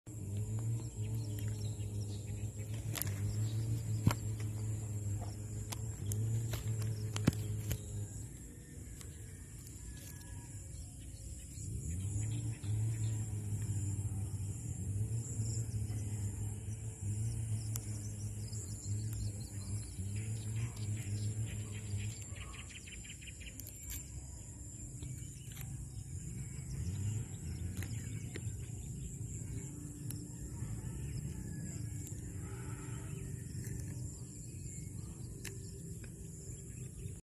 A PLACE TO RELAX & UNWIND | JAQKS VIBES
It's so relaxing walking around here. The view and the different sounds of God's creation makes me feel calm and stress-free.